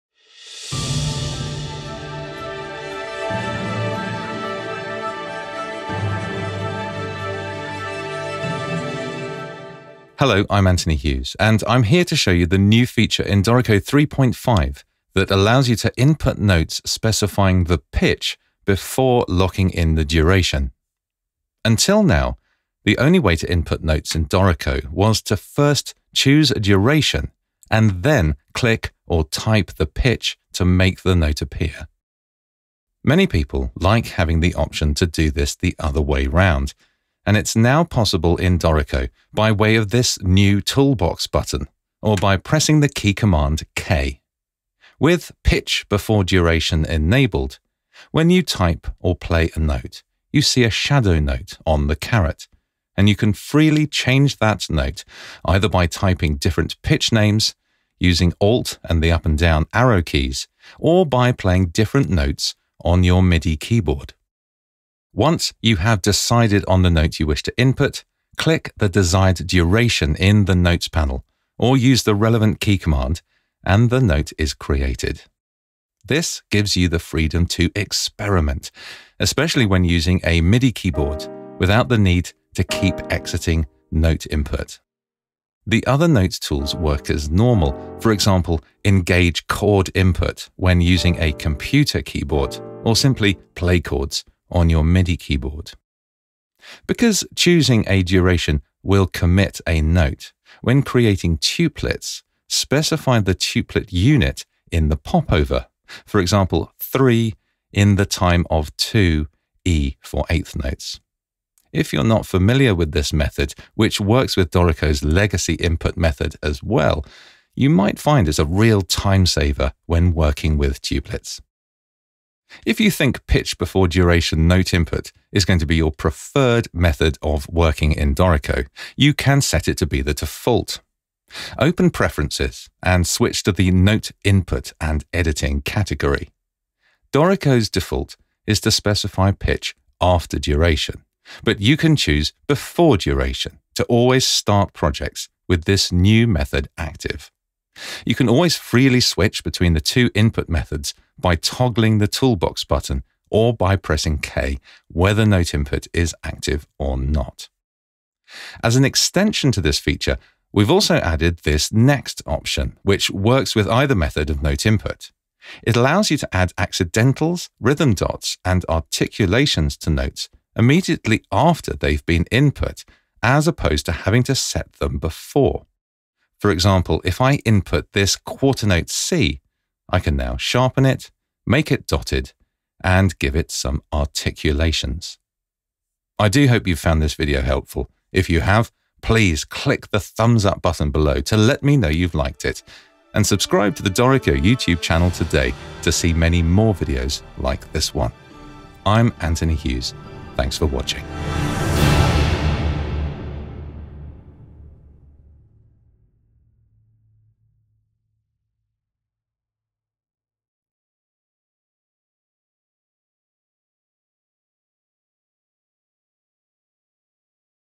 Pitch Before Duration Input | New Features in Dorico 3.5
Dorico 3.5 introduces a new way to input notes, letting you specify the pitch before locking in the duration. Learn how to enable this new behaviour and see how it helps give you the freedom to experiment and make you have the correct note before committing it to the page.